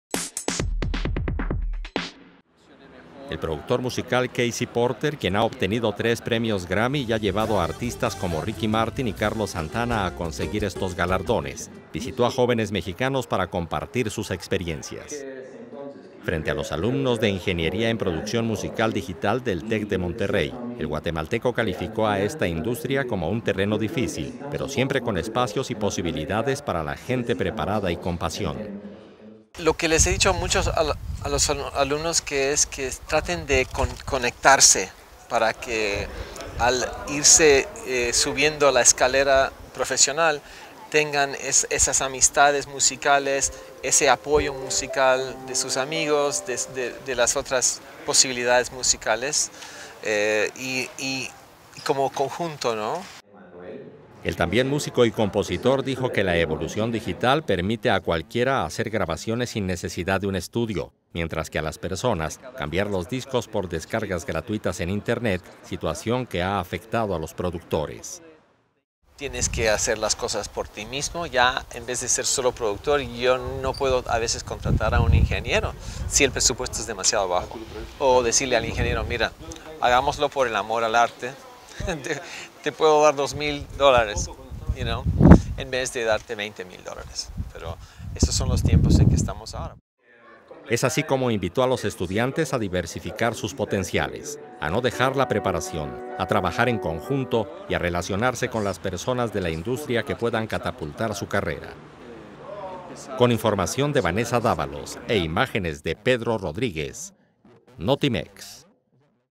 KC Porter orienta a jóvenes mexicanos
El productor musical y ganador del Grammy visita a estudiantes del Tec de Monterrey